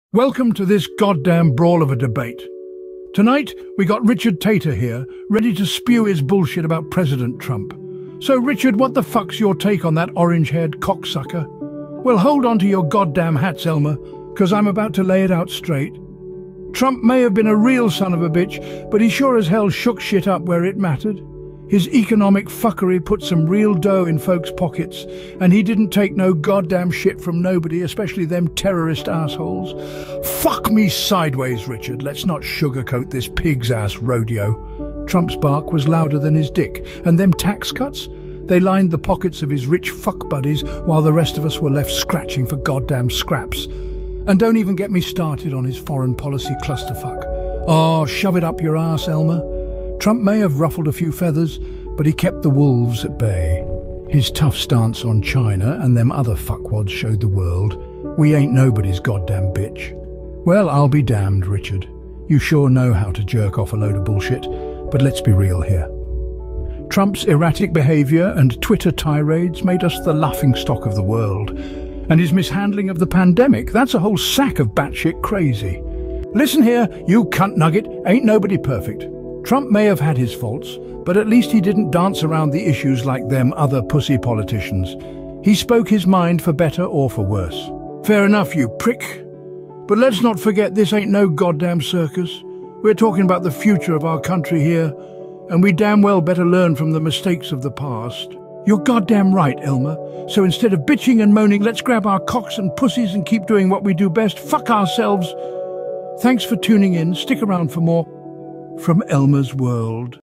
Raw and Uncensored: Trump Debate with Richard Tator and Elmer
Get ready for a no-holds-barred debate on the legacy of President Trump, featuring the fiery exchanges between Richard Tator and Elmer! 🗣️ From economic policies to foreign affairs, these two don’t hold back as they sling insults and arguments like it’s a wild west showdown. Buckle up for an unfiltered discussion filled with profanity, passion, and plenty of politically incorrect language! 🔥